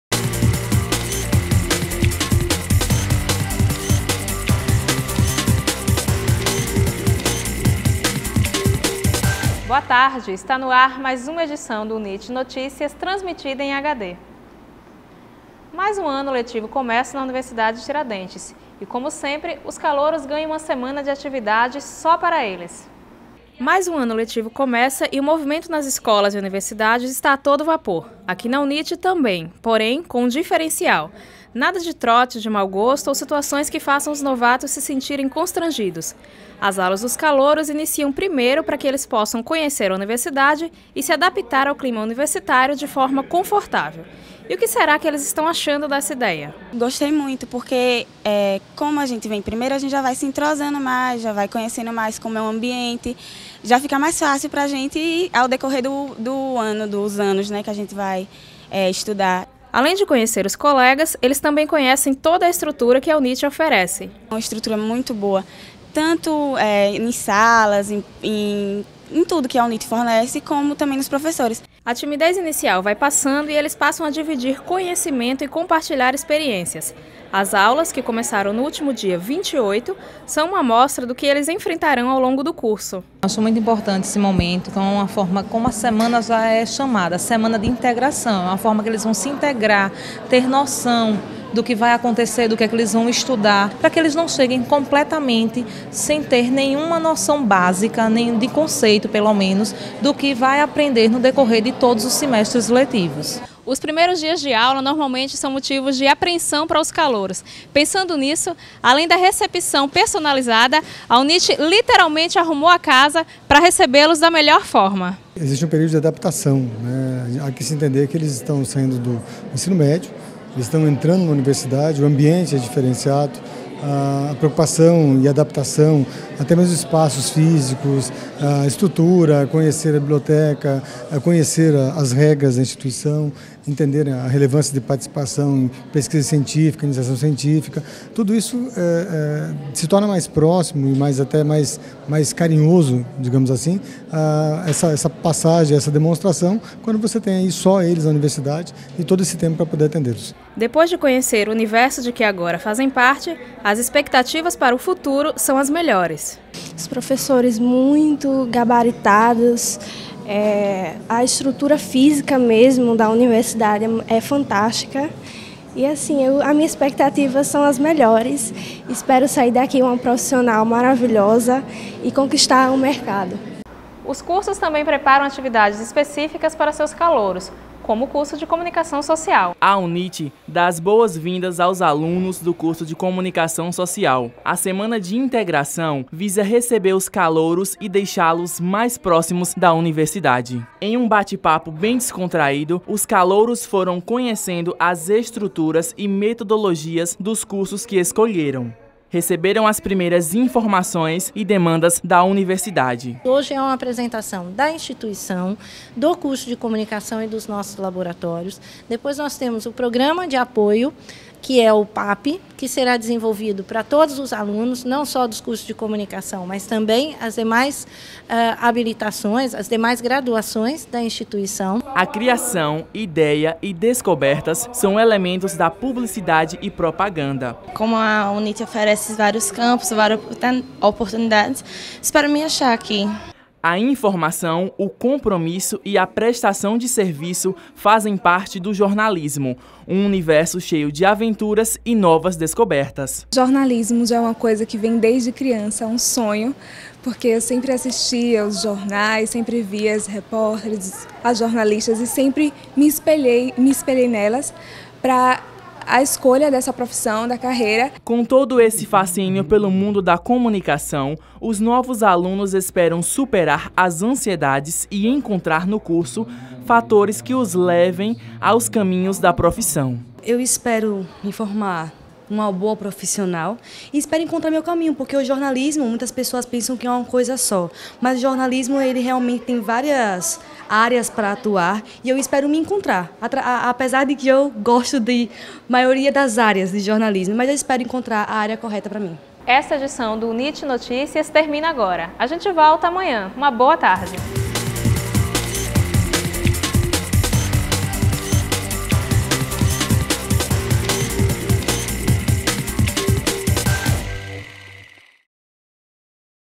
UNIT NOTICIAS 905 04 02 2013
MAIS UM ANO LETIVO COMEÇA NA UNIVERSIDADE TIRADENTES E, COMO SEMPRE, OS CALOUROS GANHAM UMA SEMANA DE ATIVIDADES SÓ PARA ELES. //

OS CURSOS TAMBÉM PREPARARAM ATIVIDADES ESPECÍFICAS PARA SEUS CALOUROS, COMO O CURSO DE COMUNICAÇÃO SOCIAL. ///